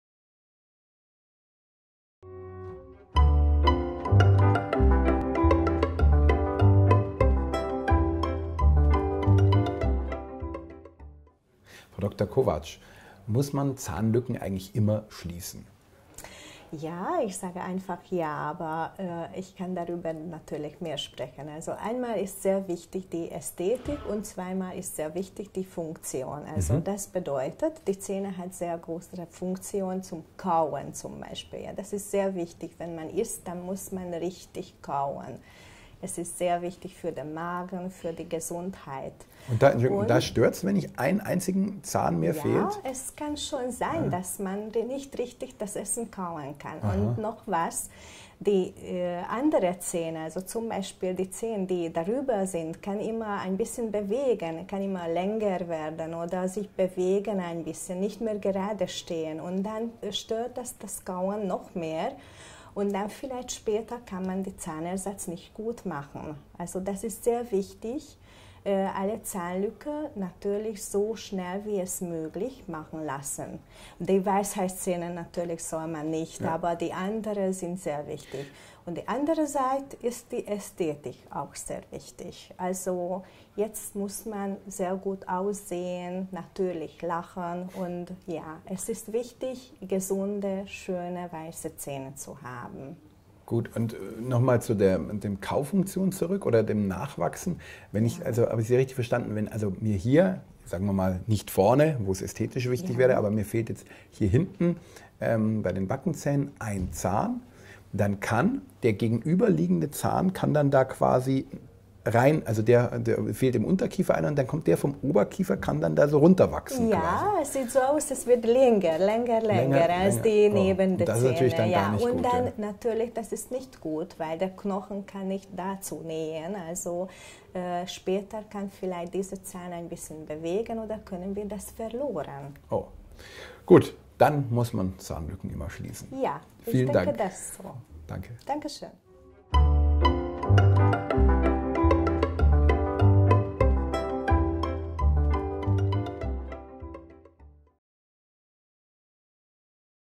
Reine Ästhetik? Warum Zahnlücken auch aus funktionellen Gründen geschlossen werden sollten!
In diesem Vlog fragt Andreas die Donau Dental Zahnärztin Frau Dr. Kovács, warum eine sofortige Schließung von Zahnlücken unumgänglich ist. Abgesehen von der Ästhetik können Zahnlücken zu einem gestörten Kauverhalten führen: So stellt sich bereits bei einem fehlenden Zahn nach einiger Zeit eine falsche Kaugewohnheit ein, welche auch negative Auswirkungen auf die Verdauung haben kann. Außerdem können Zahnfehlstellungen der Zähne, welche die Lücke umschließen oder über ihr liegen, spätere Behandlungen für einen Zahnersatz erschweren. Man kommt also nicht drum rum: Eine Zahnlücke muss schnellstmöglich geschlossen werden!